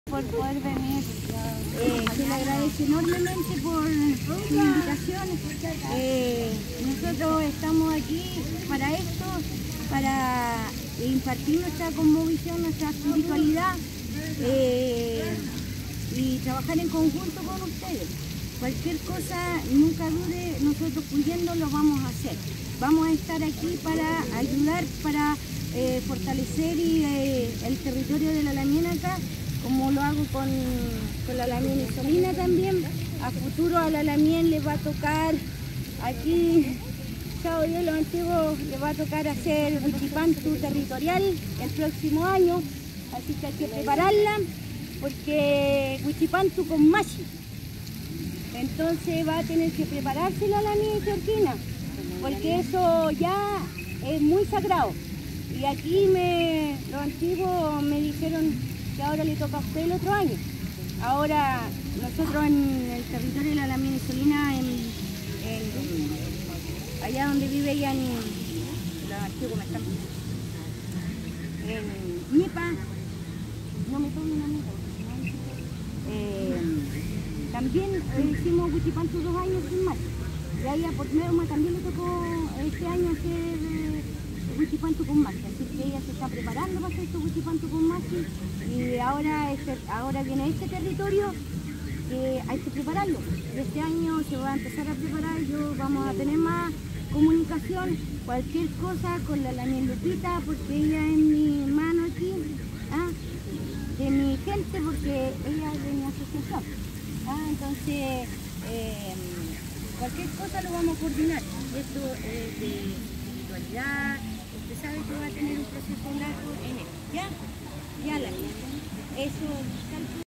Pequeña rogativa mapuche en Plaza de San Carlos - Vídeo 2
Tras el izamiento de la bandera mapuche Wenufoye en el edificio de la Delegación Presidencial de Punilla en San Carlos, delegación mapuche realizó una pequeña rogativa frente al foye, árbol del canelo, plantado hace un año. Publicado por @sancarlosnoticias